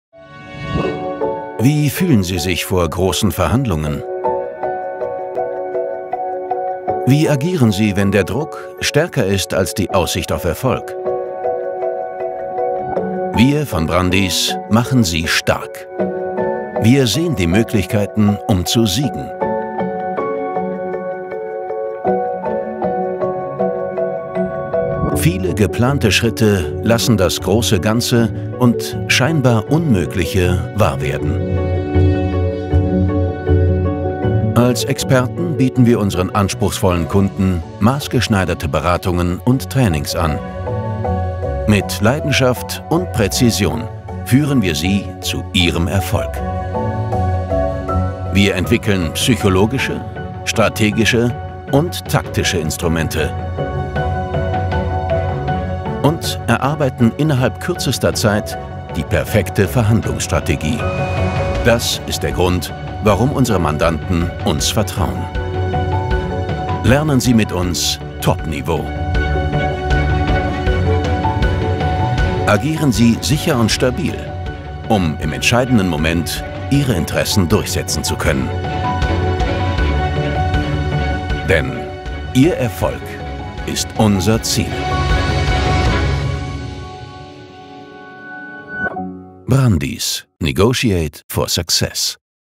Imagefilm Brandis Negotiations
Als eines der führenden Beratungsunternehmen für Verhandlungslösungen geleitet die Bradis Negotiation zielorientiert durch jede Phase schwieriger Verhandlungen und steigern die Kompetenzen ihrer Kunden in Intensiv-Verhandlungstrainings. Sie entwickeln als Verhandlungsexperte Strategien, Lösungen und Wege, die Kunden genau dorthin bringen, wo Sie hinwollen. Vertrauensvoll, persönlich und effizient.

In diesem Dreh war ich als Darsteller involviert und es ging um Unsicherheit, Vertrauen gewinnen und mit Sicherheit in die Verhandlungen gleiten.